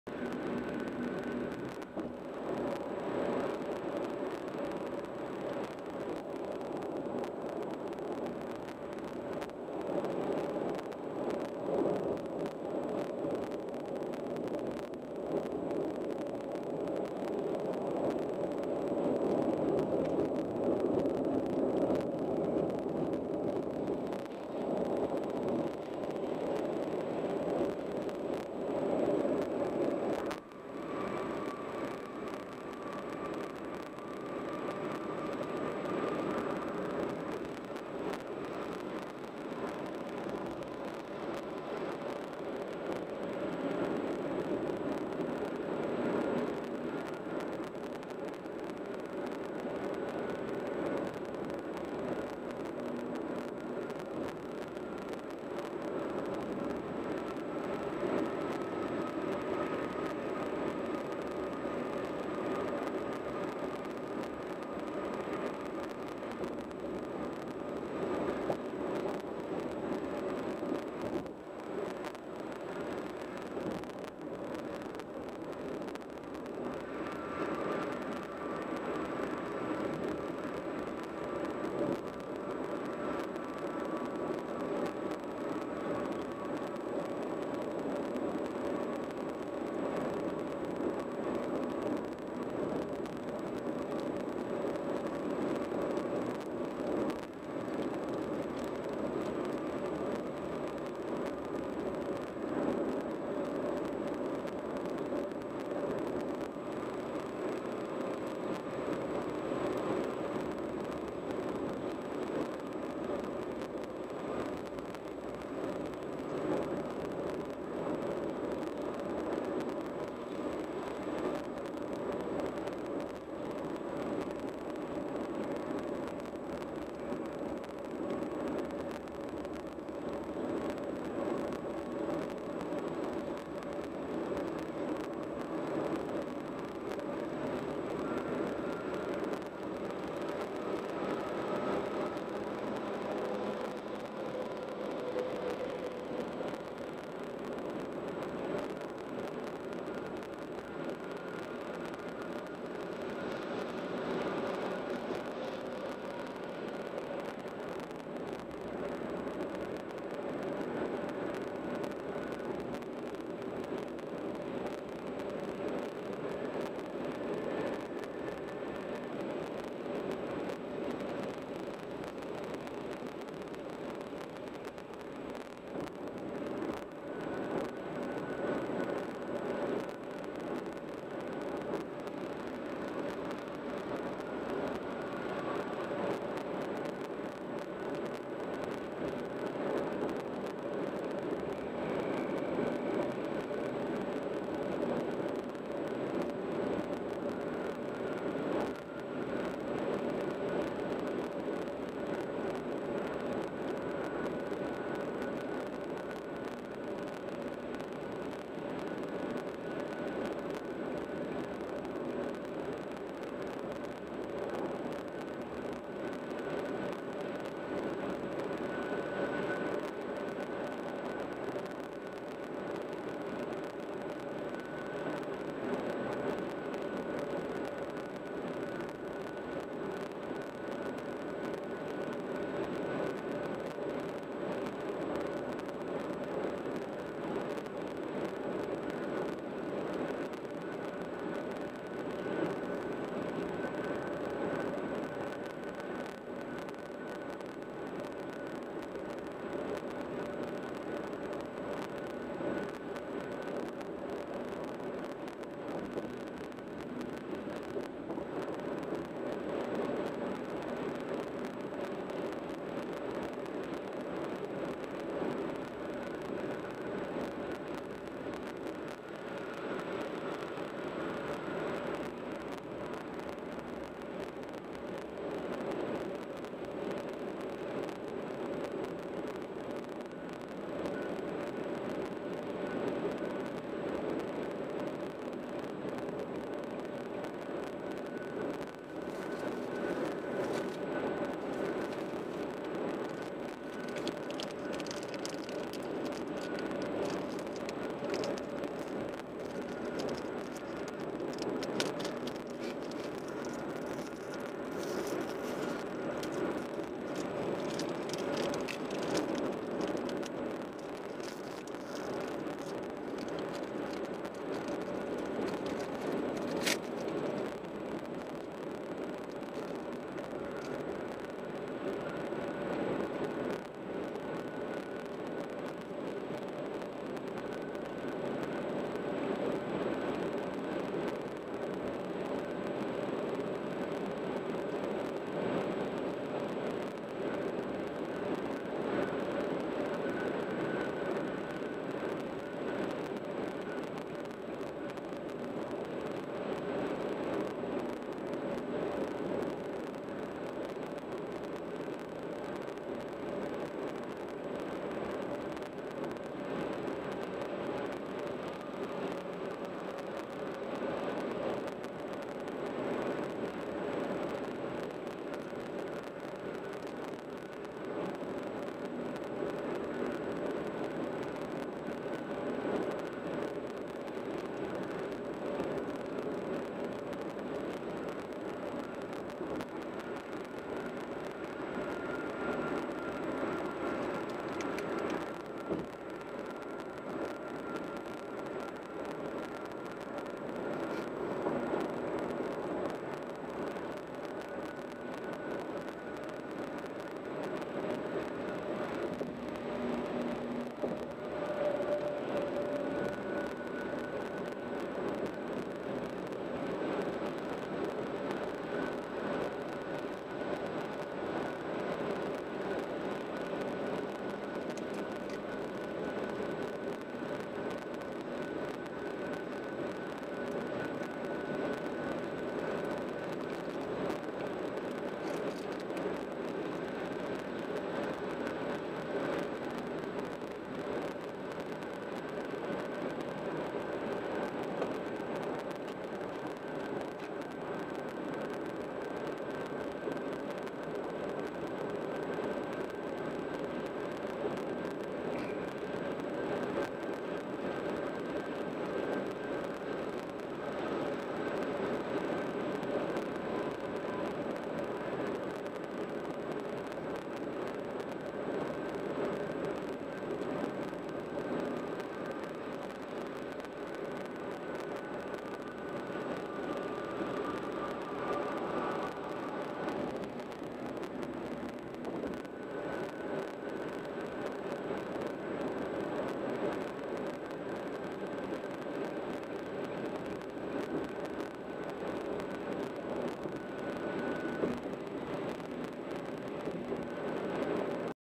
Interstate 35E - Texas (Exits 370 to 381) northbound
Across the Dallas-Fort Worth Metroplex, Interstate 35E is the main north-south interstate serving central Dallas and the eastern half of the region.